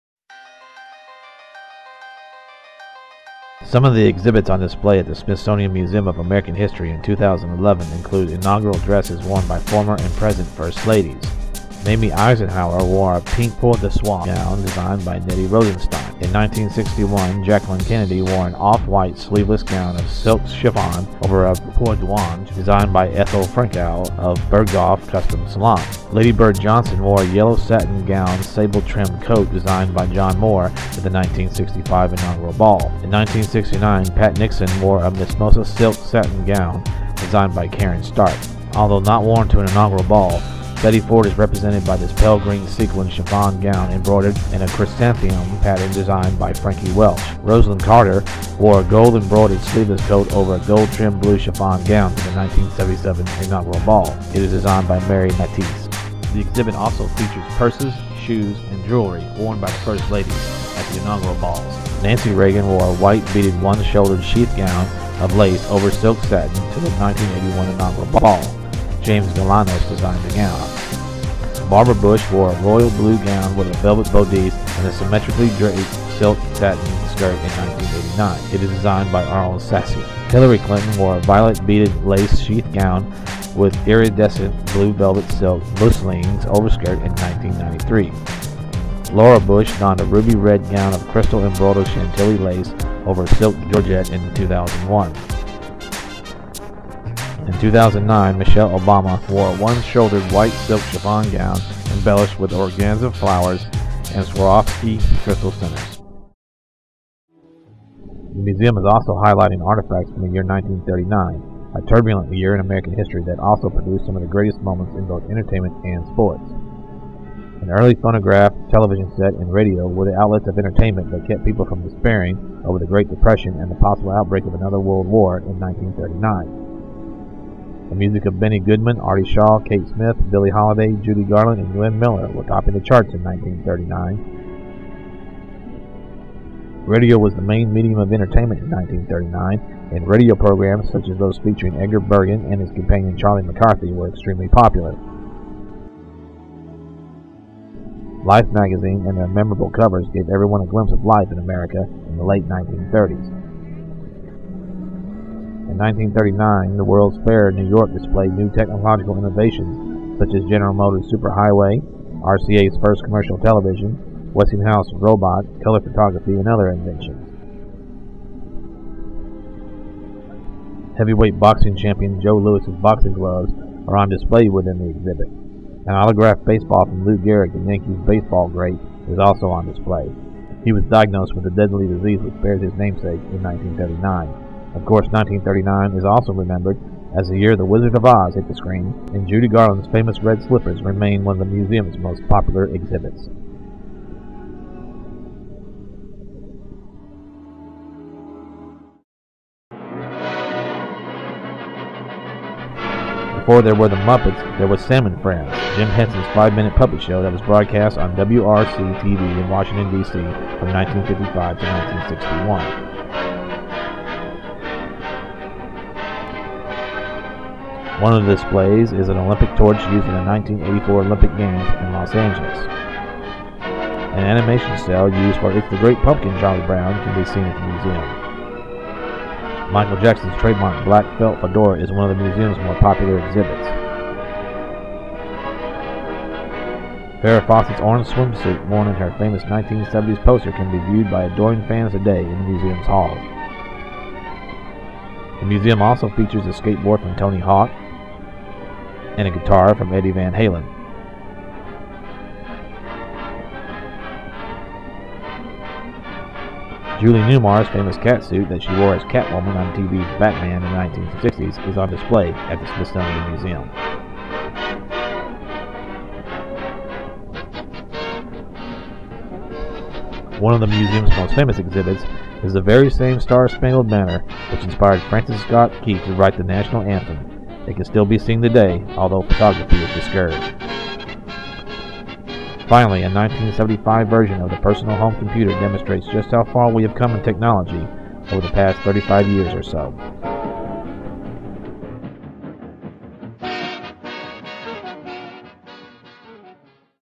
The Smithsonian's National Museum of American History Revisited
I visited the Smithsonian National Museum of American History in 2009 and went back to look at a few more things I missed the first time in 2011. This is the video I took there. I also did the narration.